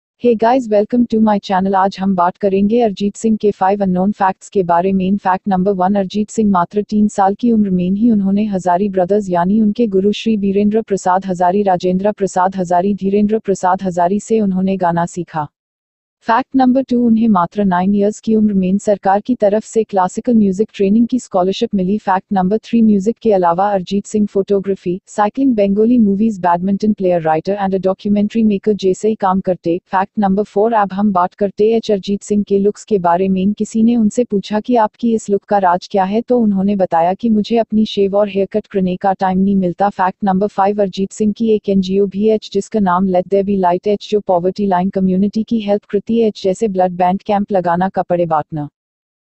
Arjits Singh Top 5 unknown facts
Hey guys this is my first fact video about Arjit Singh if u want another persons unknown fact then comments down below please like share subscribe my channel to motivate me alot thnkuuuu
Thankyou